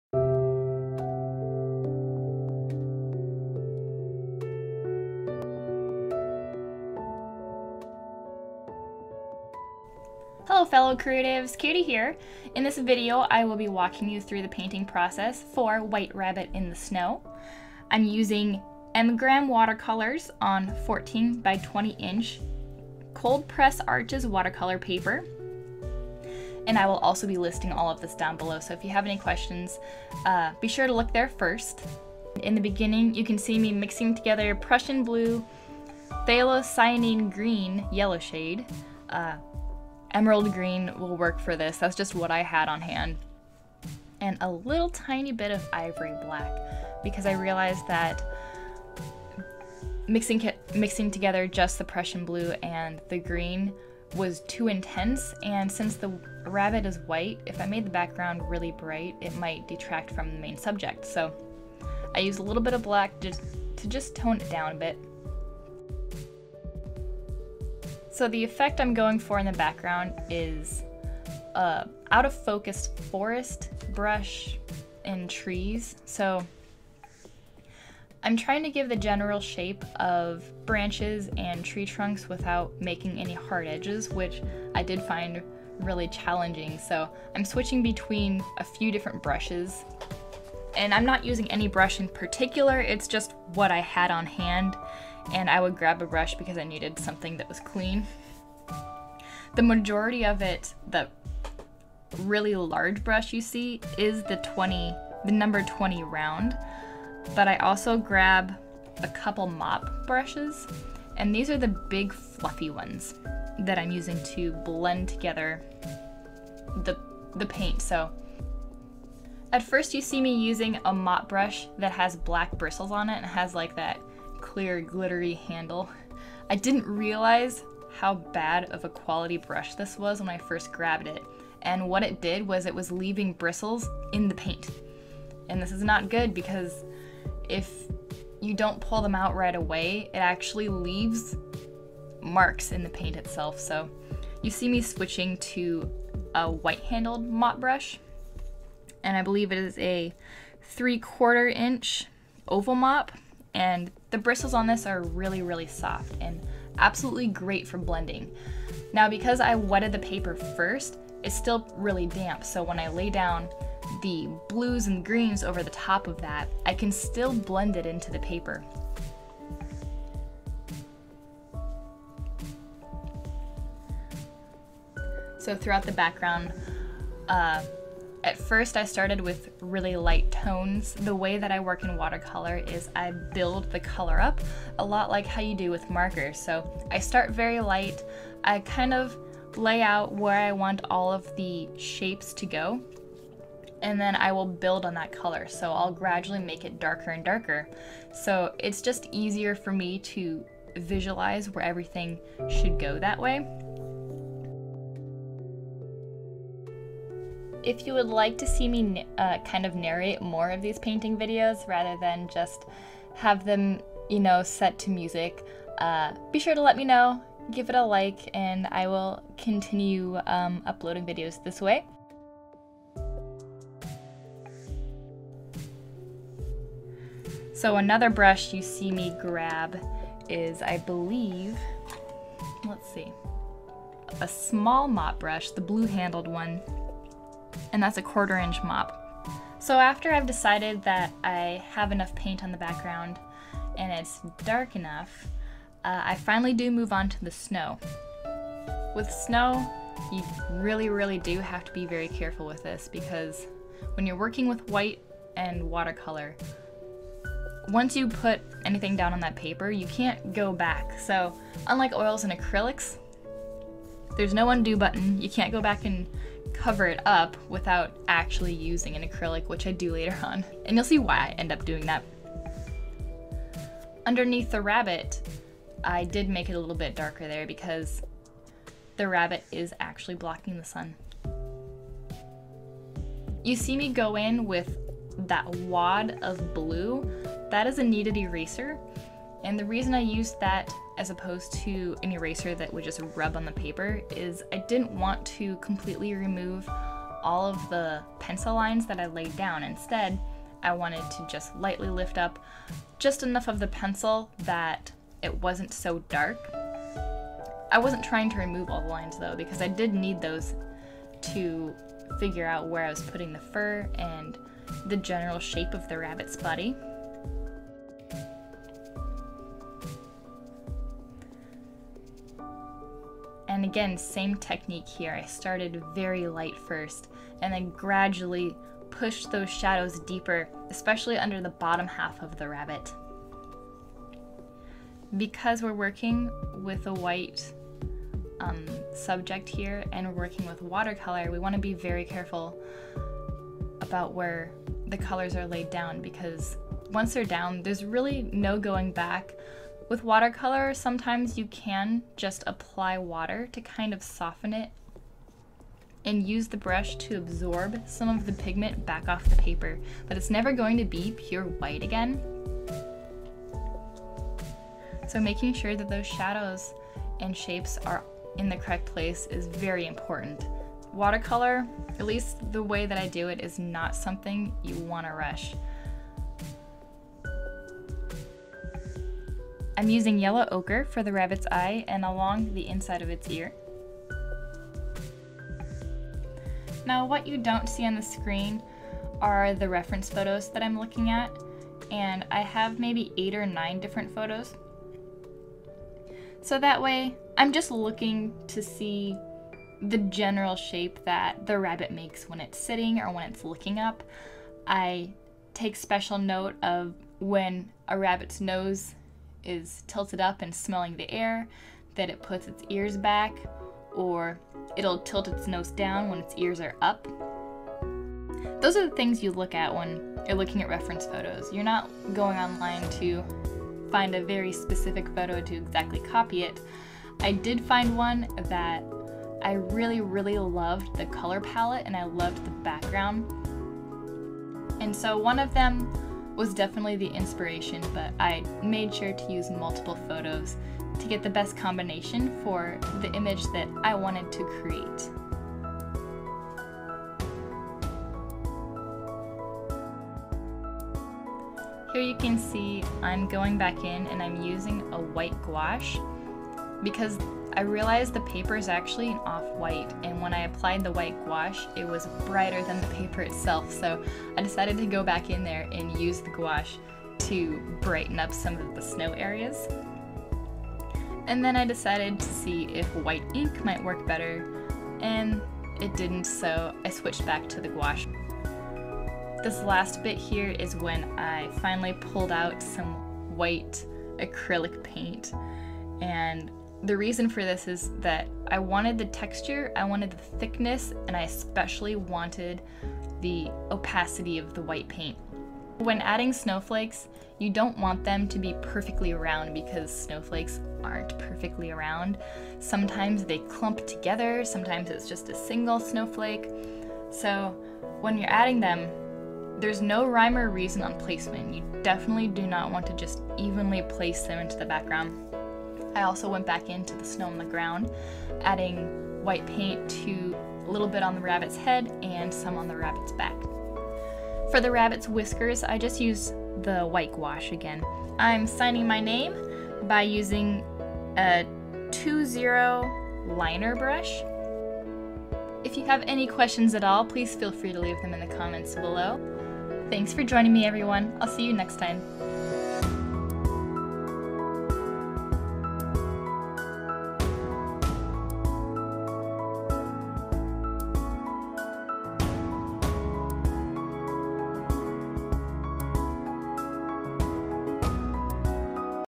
Watercolor Painting "White Rabbit in the Snow"
- Arches 140 pound cold press watercolor paper 20x14 inches
- M. Graham Watercolor paints
- Round brushes #20, #6, #2
- Mop brushes in 3/4" and 1/4"
- Liner brush 2/0 (for signature)
- White Signo Uni-Ball Gel Pen
- White acrylic paint (the snowflakes and snow textures)